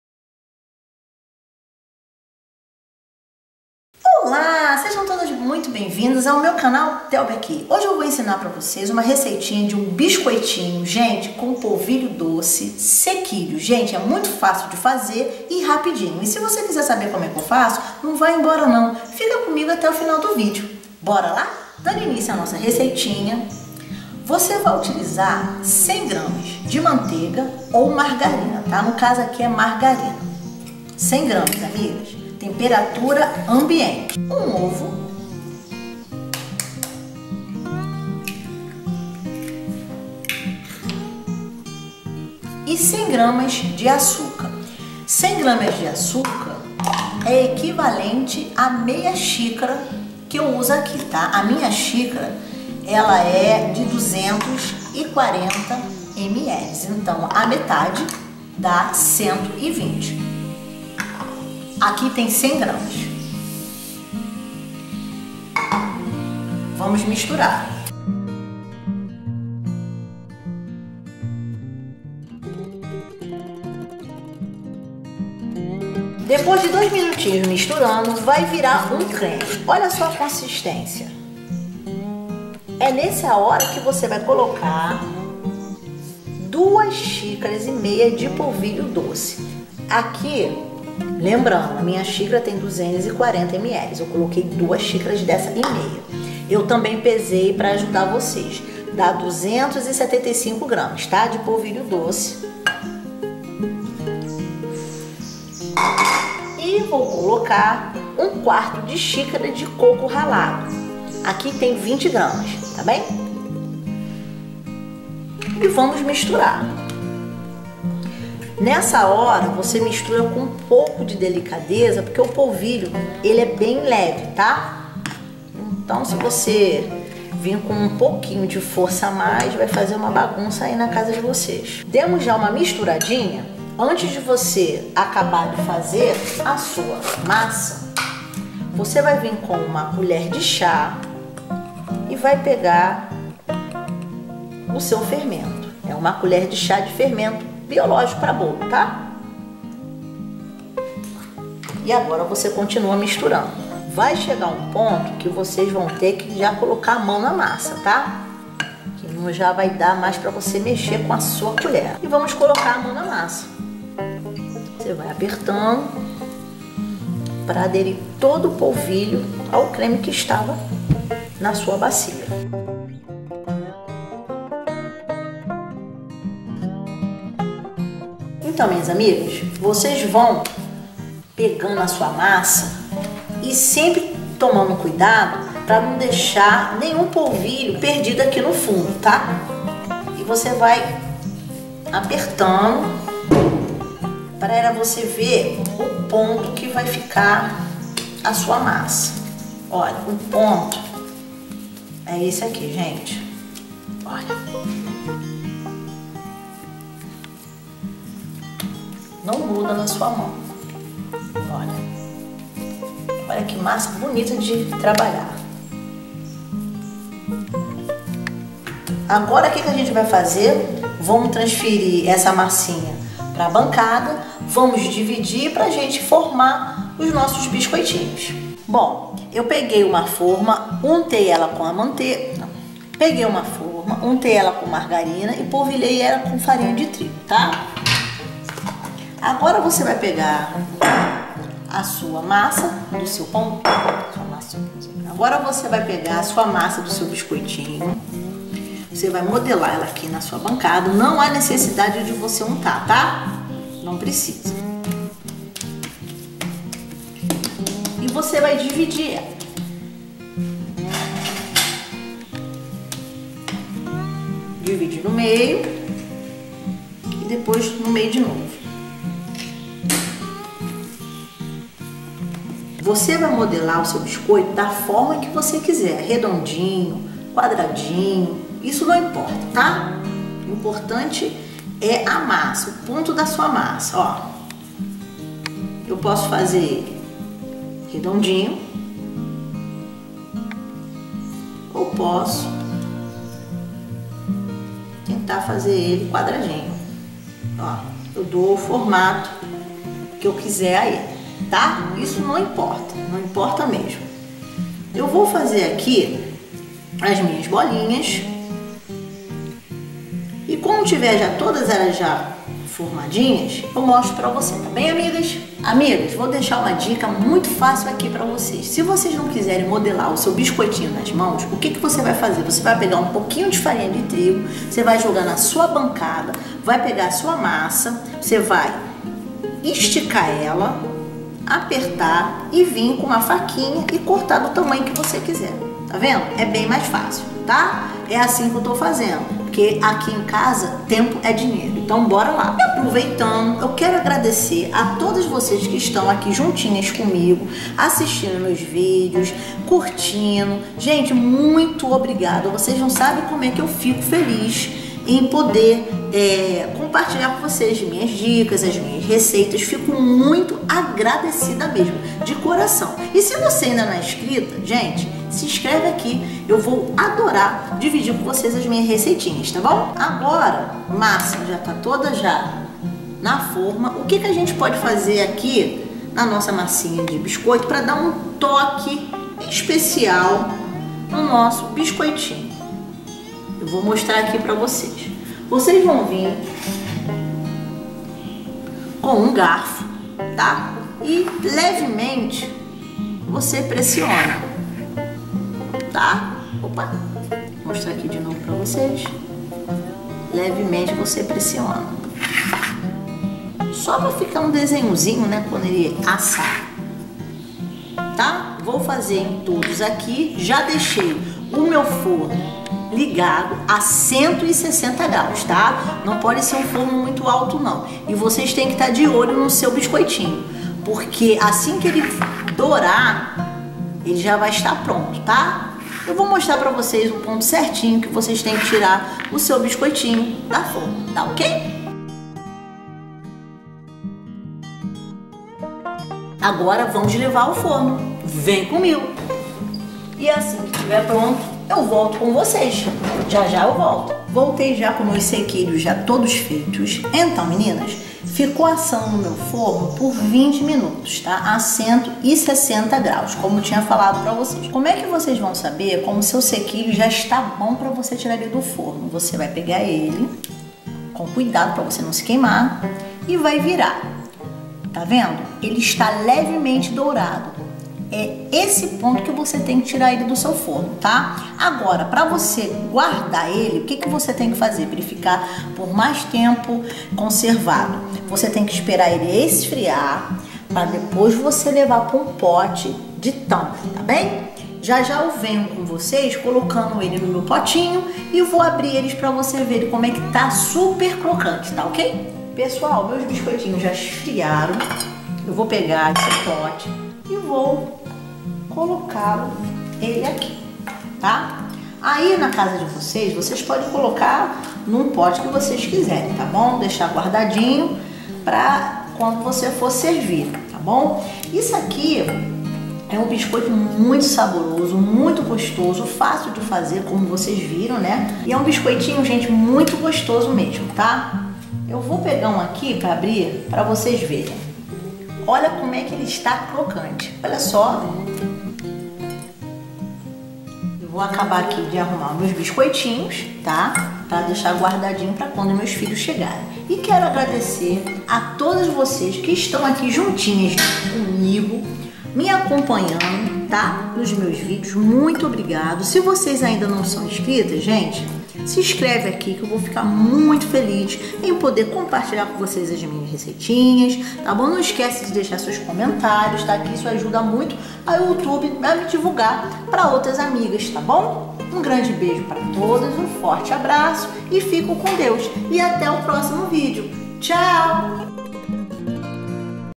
SEQUILHOS DE POLVILHO DOCE [DELÍCIA QUE DERRETE NA BOCA]
Maravilhosa receita de biscoito de polvilho doce.
Uma receita fácil e prática de ser feita, com poucos ingredientes.
Canal Telbia Aqui recitas fáceis e rápidas de fazer.

INGREDIENTES

1/2 XÍCARA DE CHÁ DE MARGARIGA (100 GRAMAS)
1 OVO
100 GRAMAS DE AÇUCAR REFINADO
2 XÍCARAS E MEIA DE POLVILHO DOCE (275 GRAMAS)
20 GRAMAS DE COCO RALADO (1/4 DA XÍCARA)
1 COLHER DE CHÁ DE FERMENTO EM PÓ